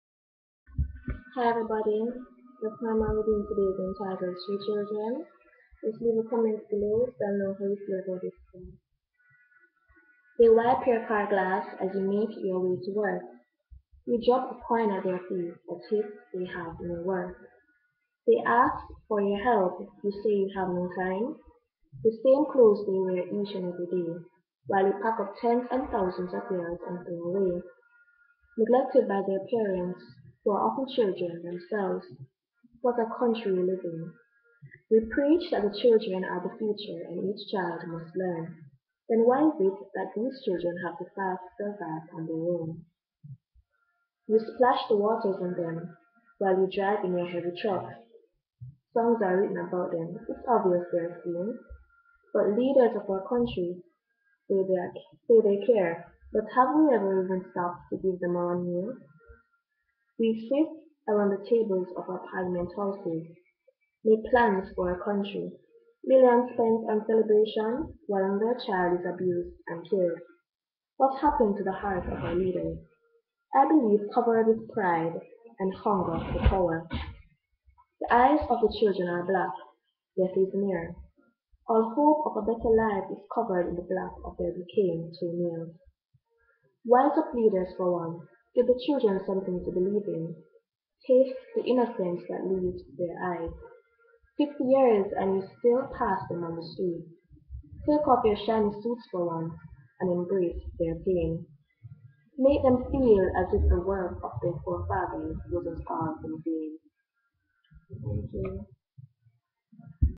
Street Children
We have all come across a child living on the street at some point or the other. This poem is to bring attention to their need for help.